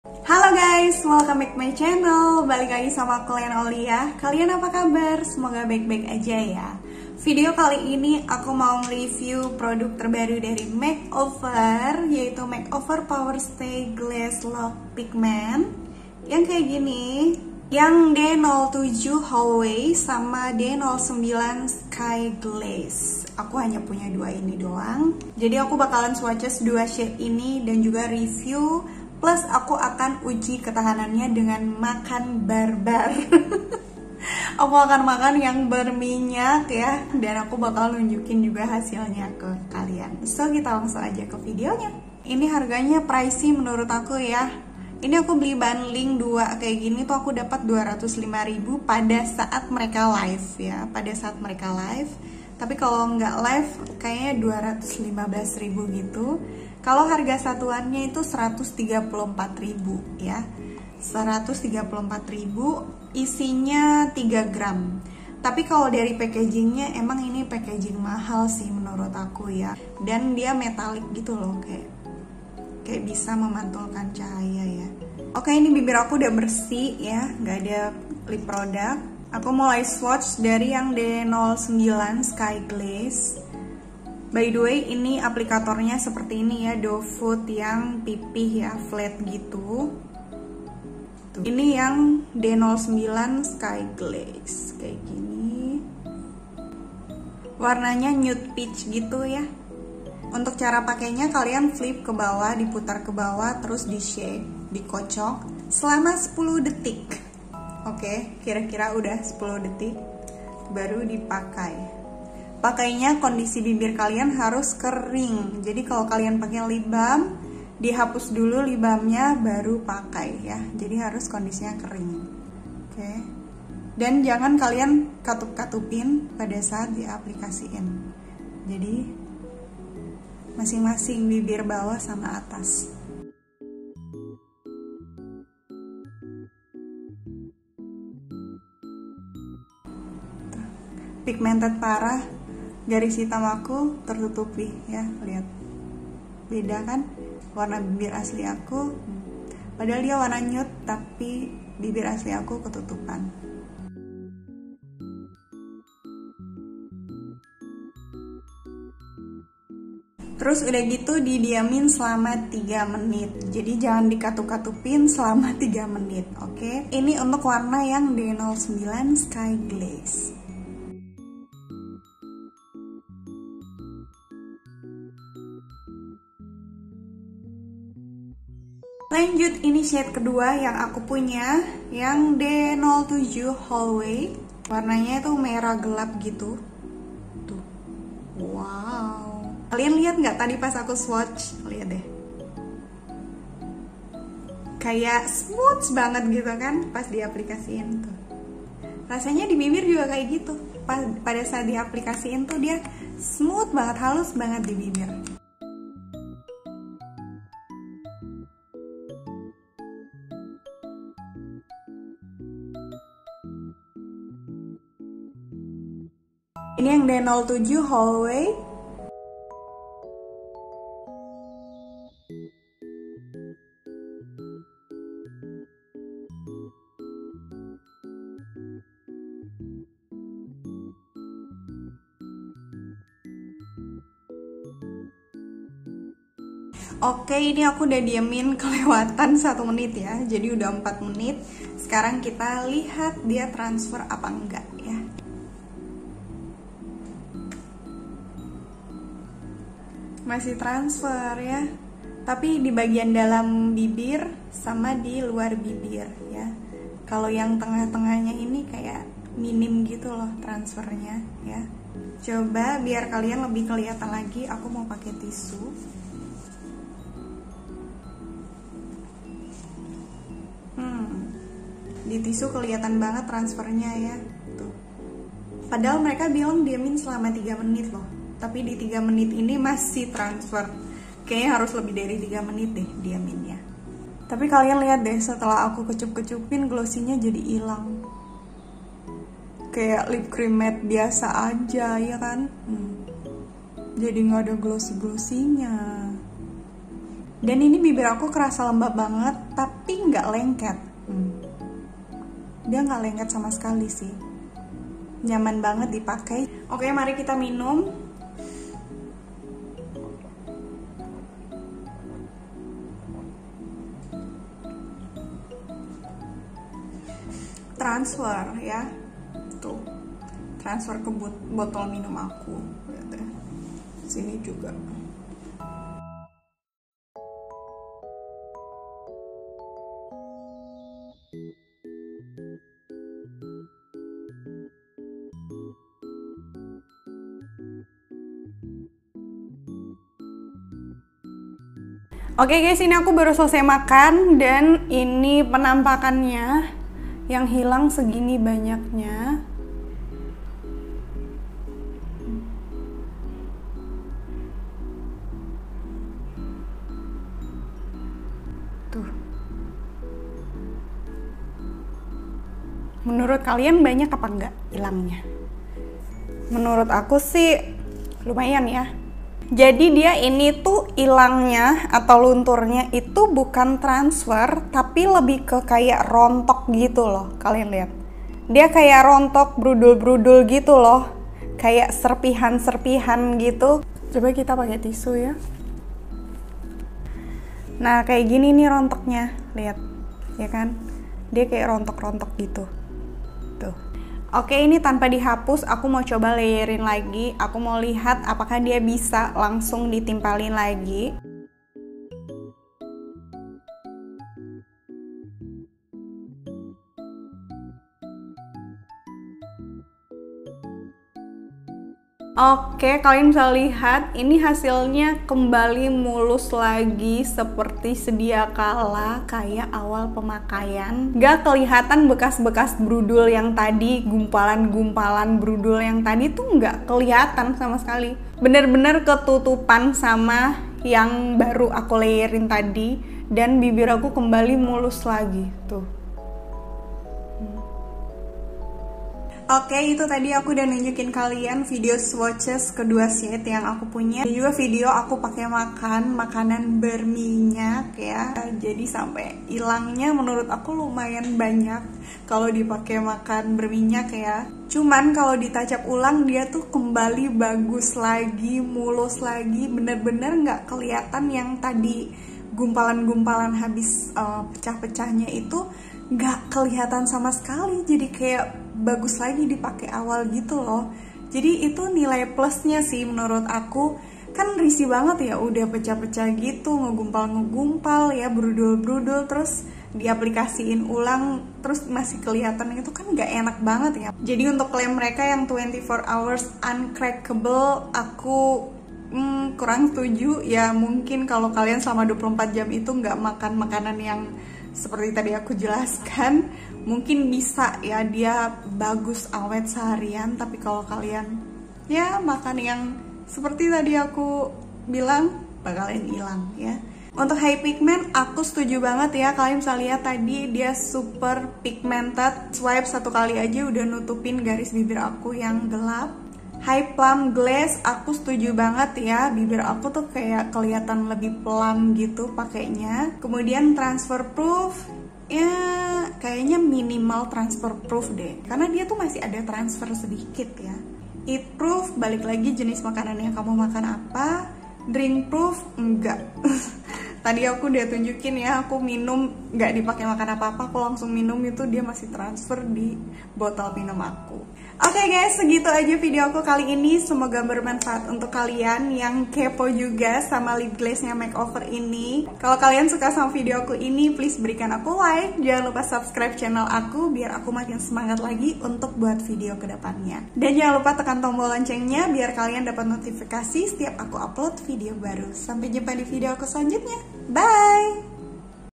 Terbaru! Make over powerstay glazed lock pigment (review + tes makan berminyak)
Stay in touch with me:
Semoga video ini bermanfaat, jika kalian suka..
With love, Lenaulia Rosres 💋
Kode: XDAK3T

Produk makeup yg dipakai:
💕Somethinc copy paste tinted sunscreen (N02 charlote)